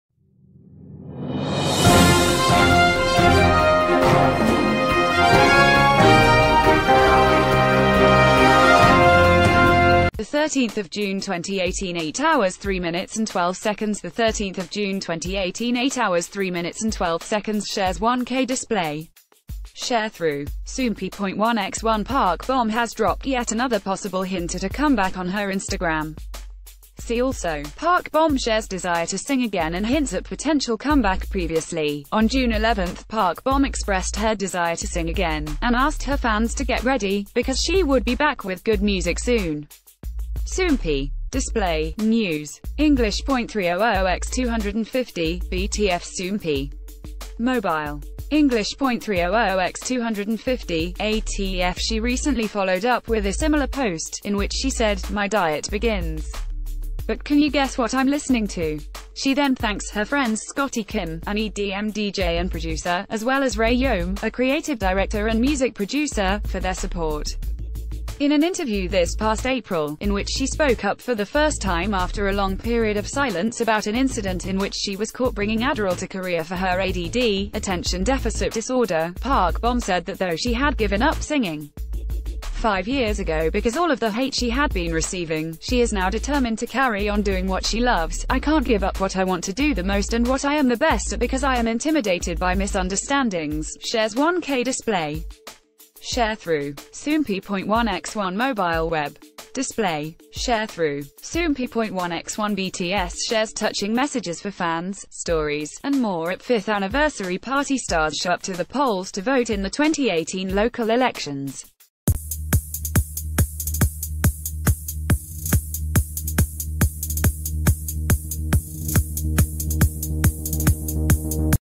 Park Bom Drops Another Possible Comeback Hint On Her Instagram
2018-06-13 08:03:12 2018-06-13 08:03:12 Shares 1K Display. Sharethrough. Soompi.1x1 Park Bom has dro...